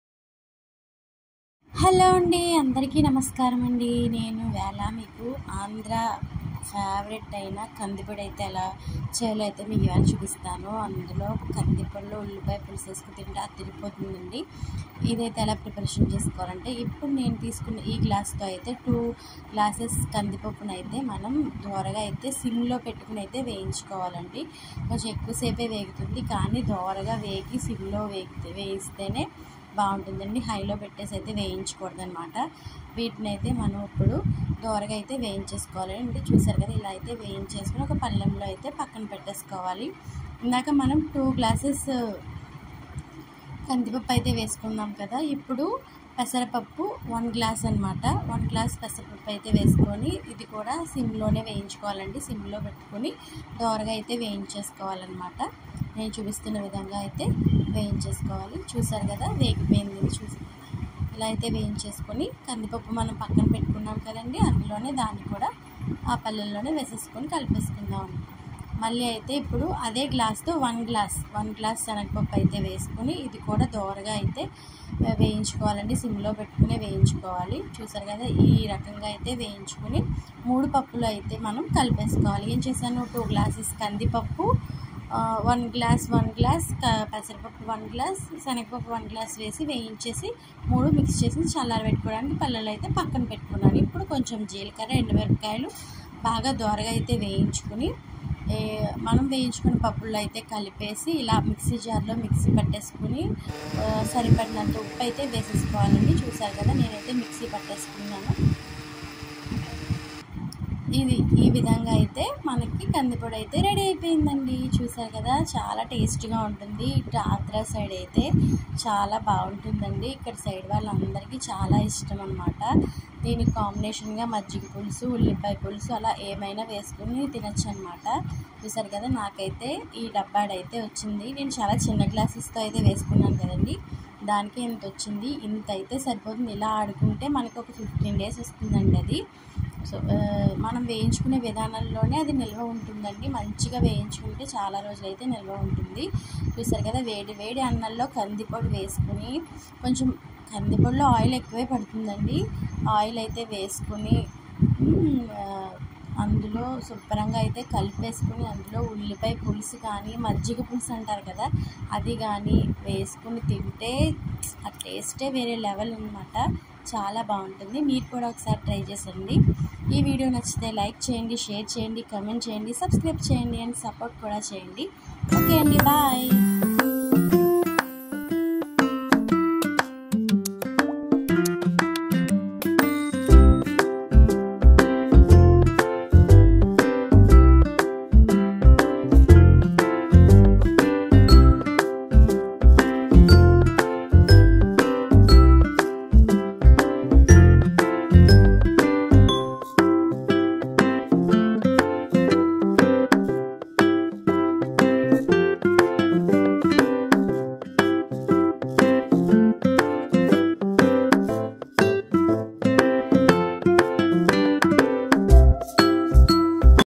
ఎంతో రుచిగా ఉండే ఆంధ్రా స్పెషల్ కందిపొడి 👌😋moreviews
ఆంధ్రా స్పెషల్ కందిపొడి,కందిపప్పు,పెసరపప్పు,శెనగపప్పు ,జీలకర్ర,ఎండుమిర్చి ఇవి ఉంటే చాలు ఎంతో రుచిగా ఉండే కందిపొడి రెడీ
how to prepare kandi podi

పెళ్ళిళ్ళలో  ఫంక్షన్స్ లో ఇది లేకుండా ఉండదు